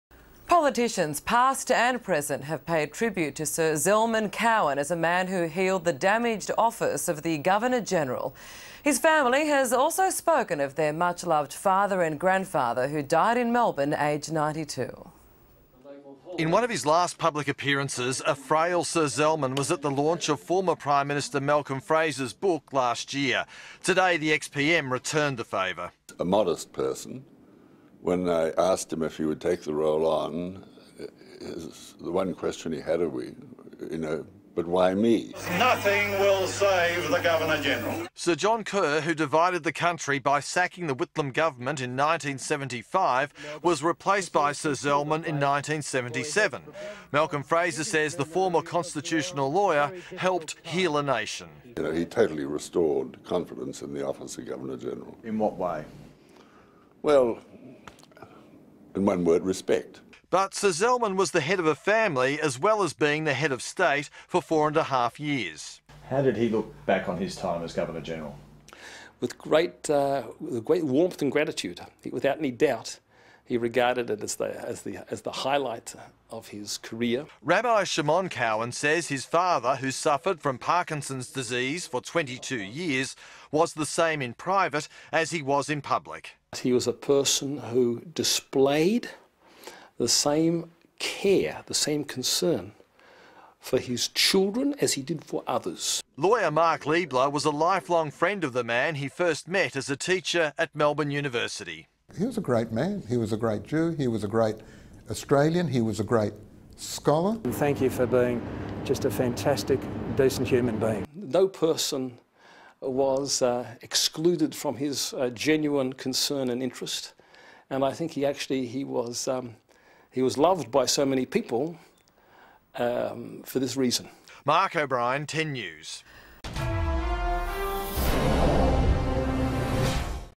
Sir Zelman Cowen 1919 - 2011
Politicians, past and present have paid tribute to former Governor-General, Sir Zelman Cowen as a man who healed the damaged office of the Governor General. His family has also spoken of their much loved father and grandfather who died in Melbourne, aged 92.  This report was broadcast on the national TV network, Channel 10 on its evening news on 8 December, 2011